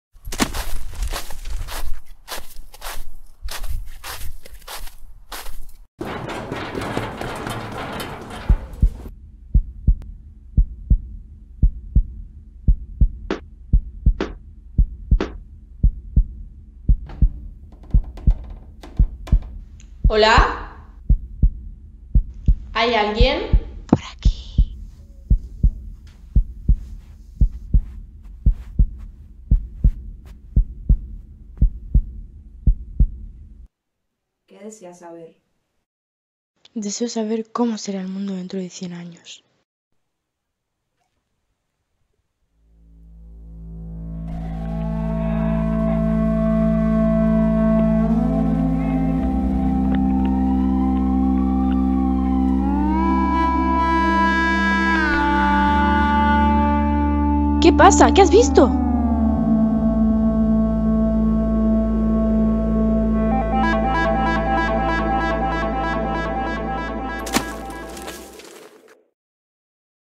Videncia Ecológica
Amaya se adentra en un misterioso lugar en busca de respuestas sobre nuestro futuro incierto.

Colegio San Agustín 4ºESO-C
Festival de Clipmetrajes de Manos Unidas